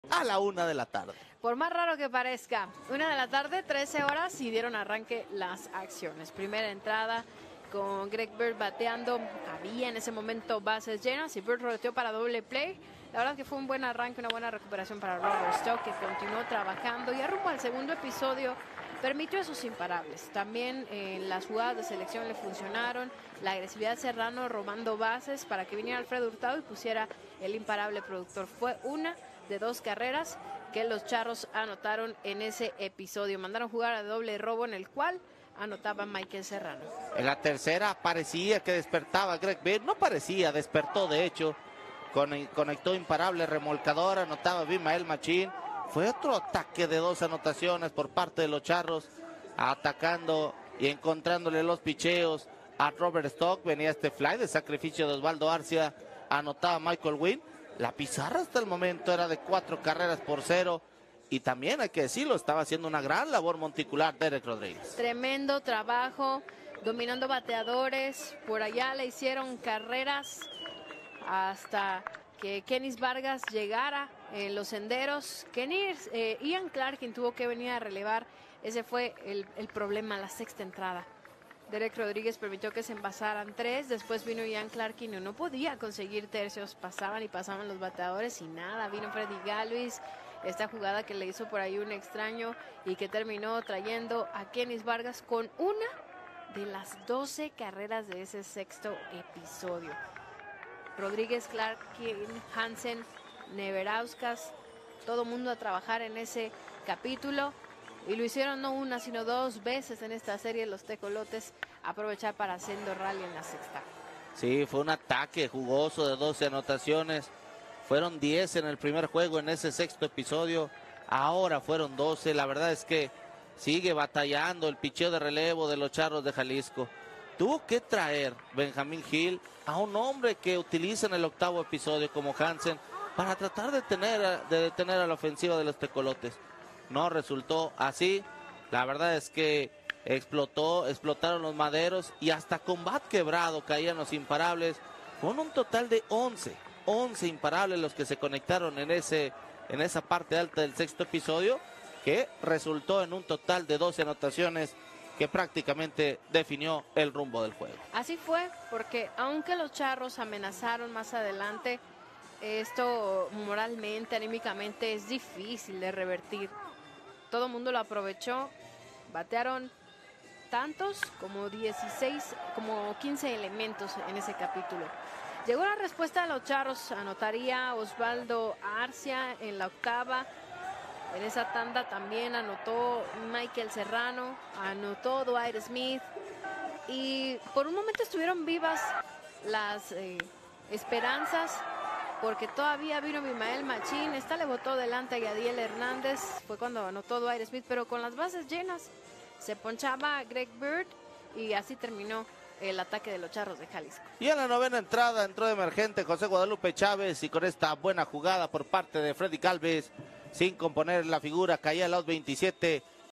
Tecos gana en el estadio Panamericano (11/07/2024)
Highlights del juego entre Tecos de los Dos Laredos y Charros de Jalisco